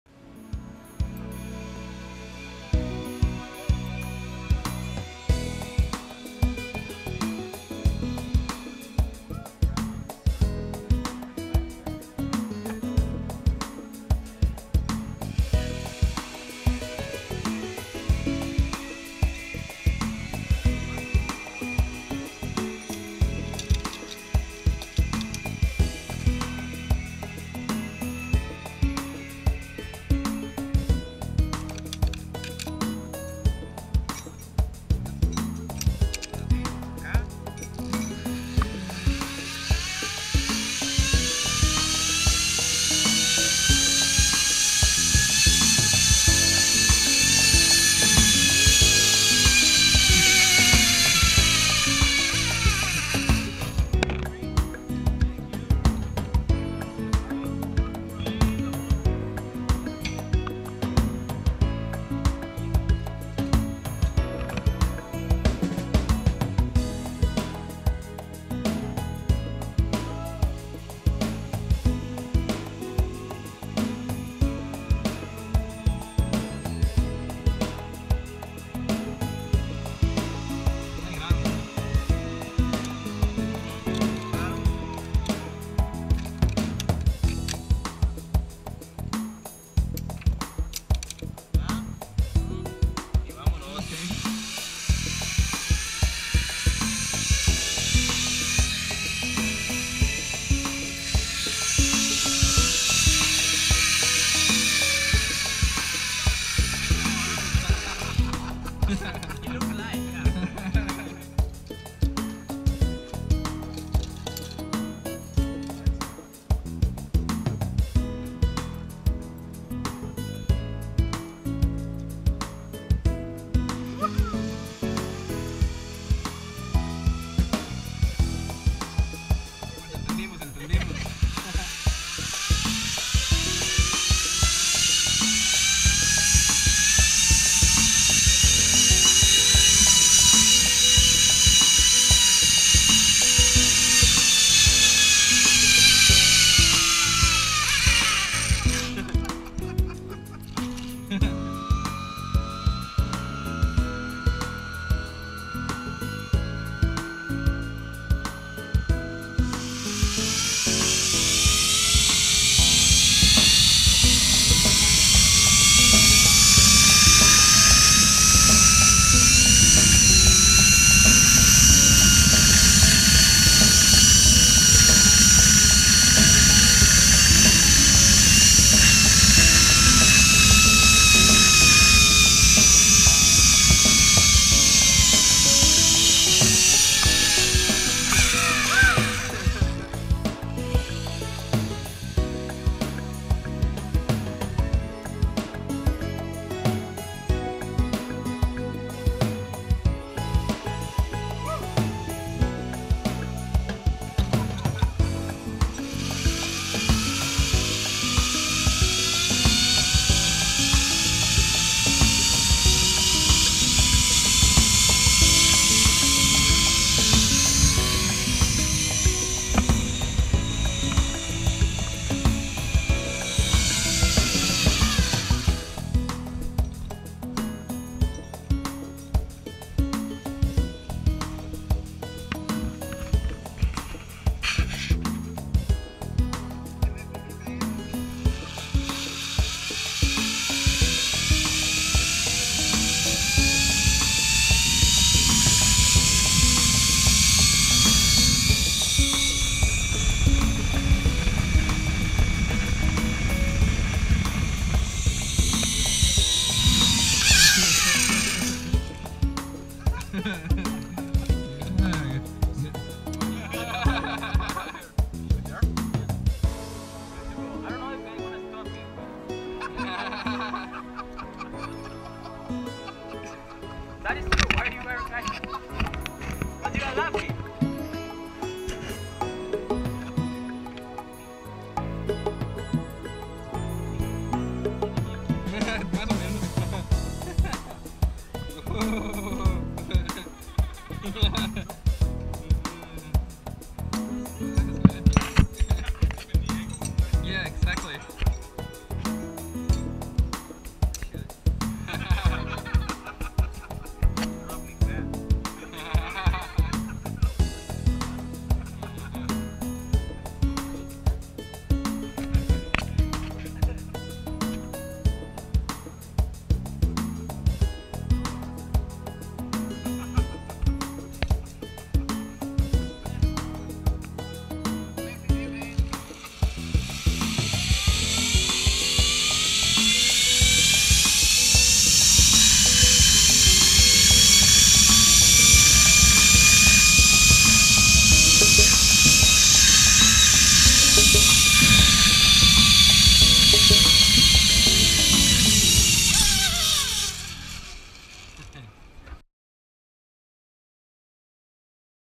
Zipline
Ziplining at MidWorld in Costa Rica